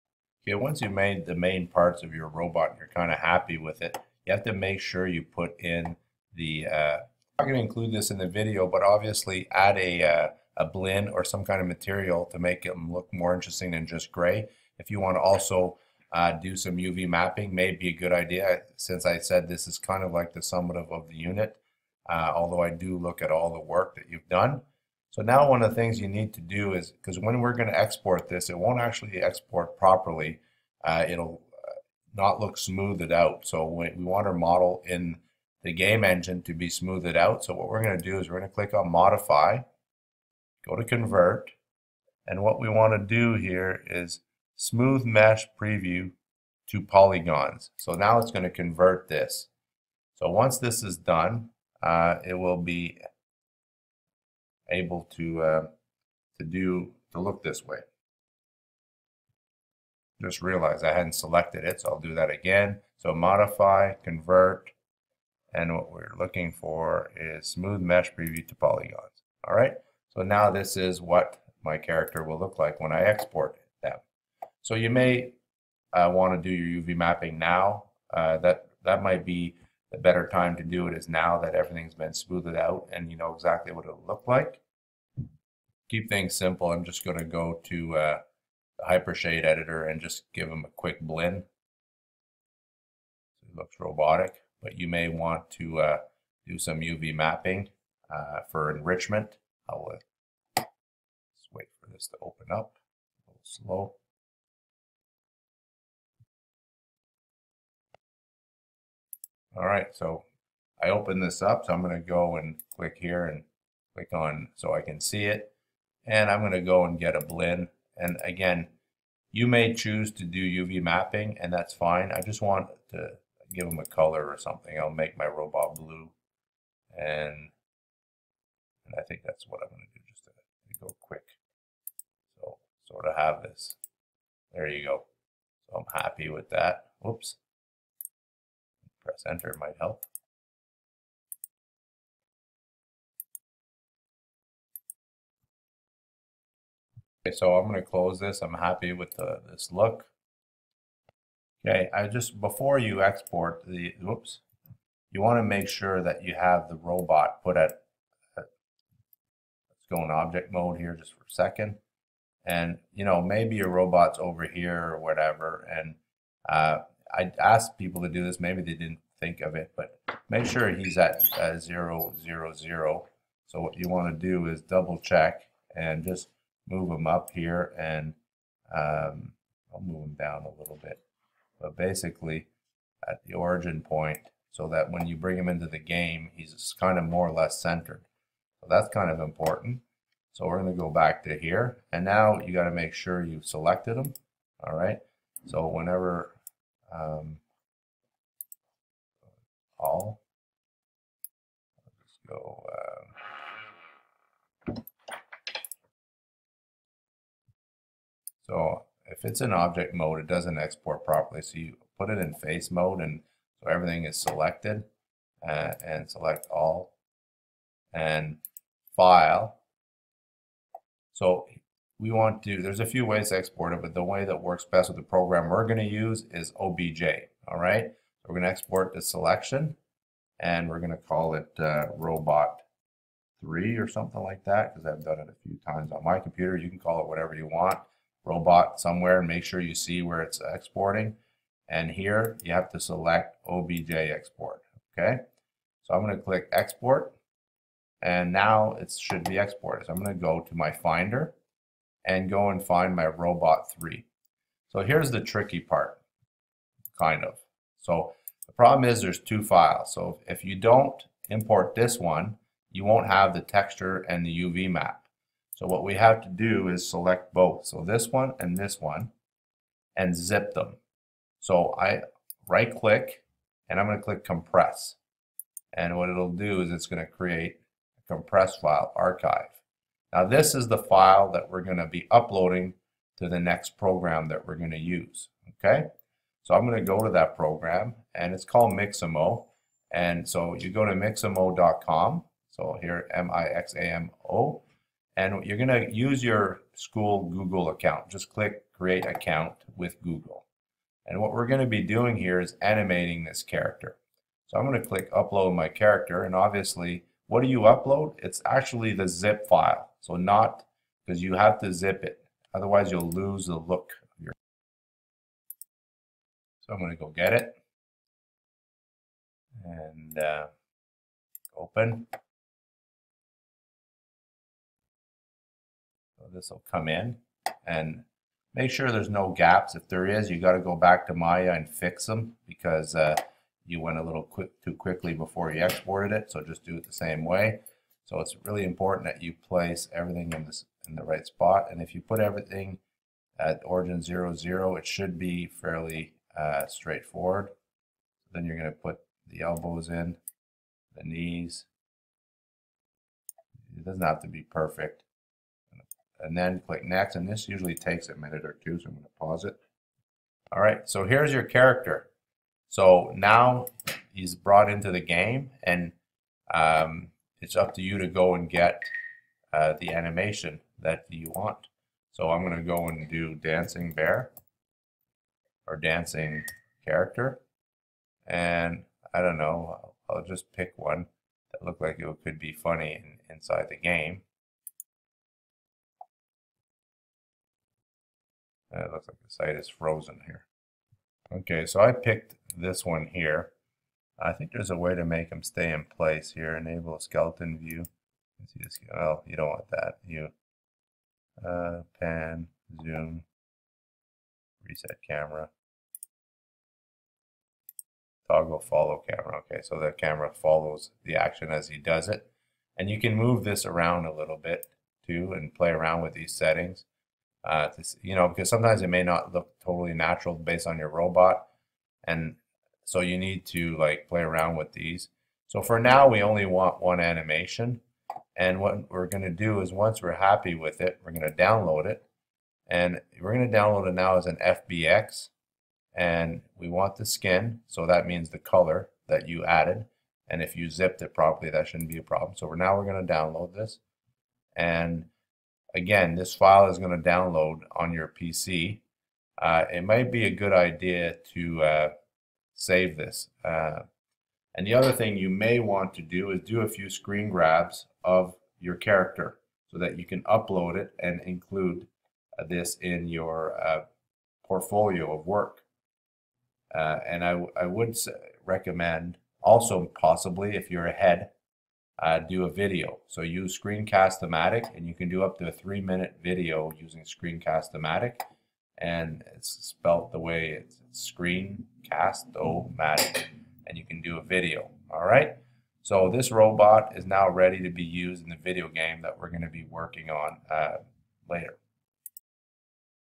Learning to Export Maya 2018 ROBOT to Mixamo for Animation
Students add final edge loops and export Model as OBJ from Maya 2018.  Import and animation to Maya